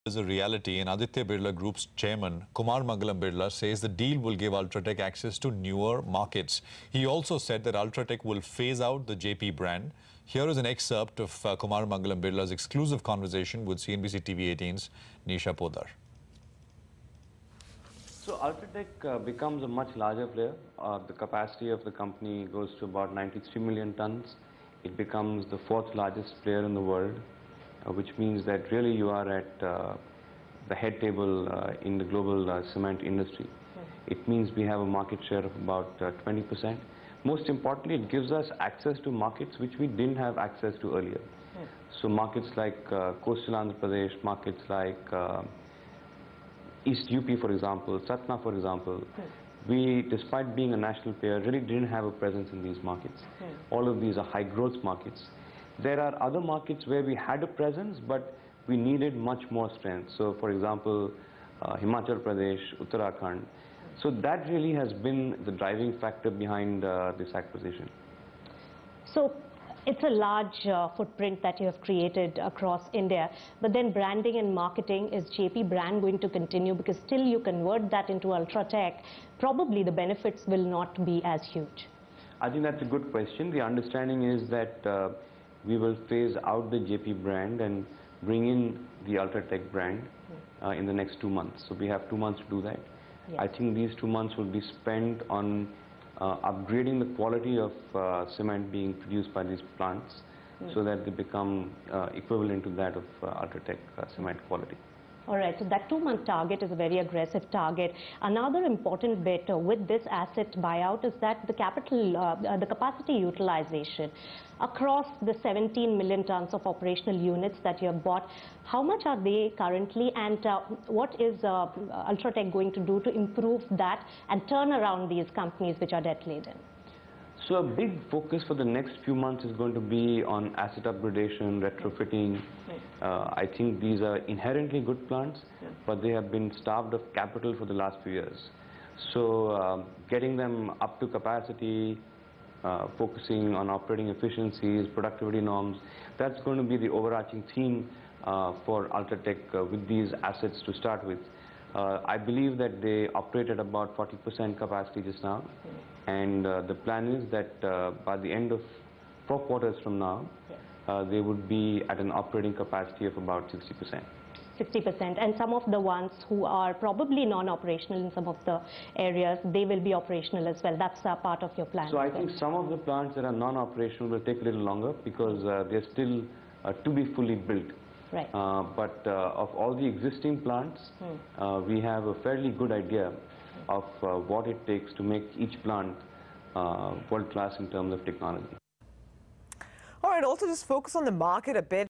UltraTech Will Become A Much Larger Player: KM Birla
KM Birla states that UltraTech will become a much larger player going ahead.